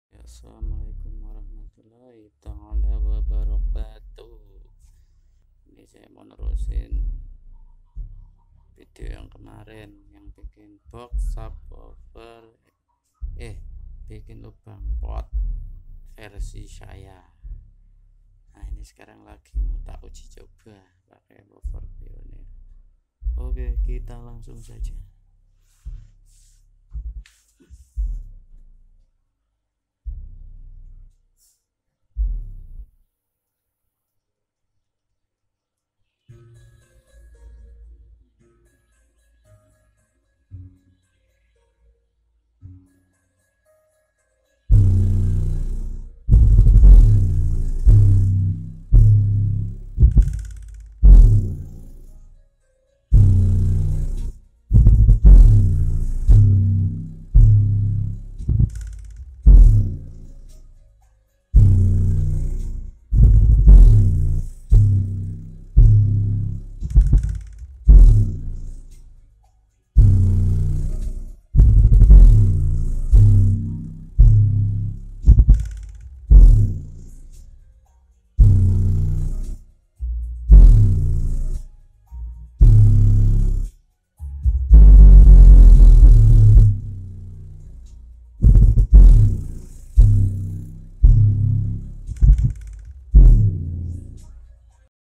TES LUBANG PORT
assalamualaikum warahmatullahi wabarokatuh,maaf temen2 baru sempet upload video hasil Tess box +port versi saya,langsung aja yg suka video nya boleh SUBSCRIBE dan Like, kritik dan saran bisa tulis komentar Salam Glerrr horeeggg Car Audio.. maaf di akhir video gelap dikarenakan Listrik Padam..😁😁😁